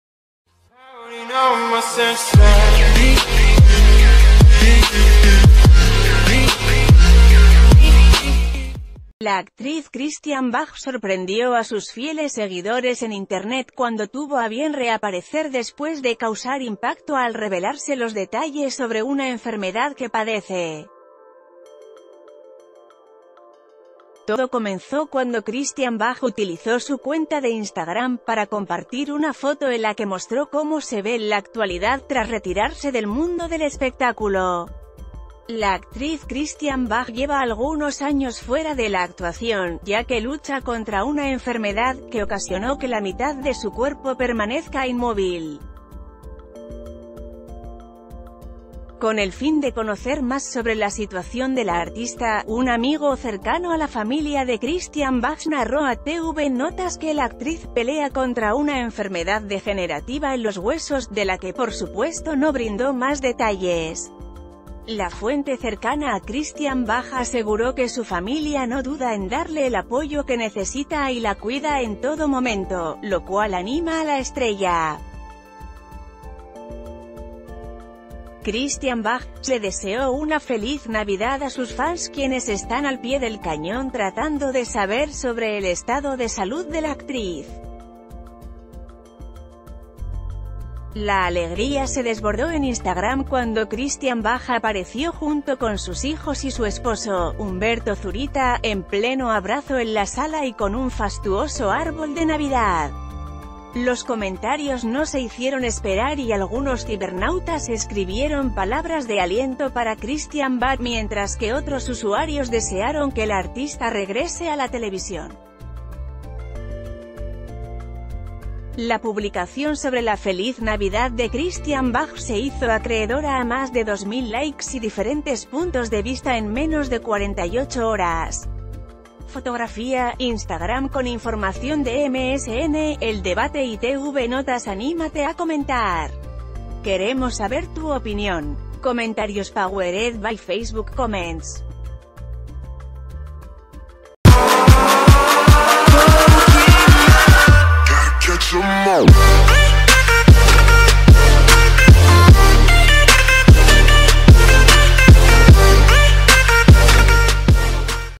Christian Bach tiene emotiva reaparición tras supuesta enfermedad degenerativa
La actriz Christian Bach sorprendió a sus fieles seguidores en internet cuando tuvo a bien reaparecer después de causar impacto al revelarse los detalles sobre una enfermedad que padece. Todo comenzó cuando Christian Bach utilizó su cuenta de Instagram para compartir una foto en la que mostró cómo s...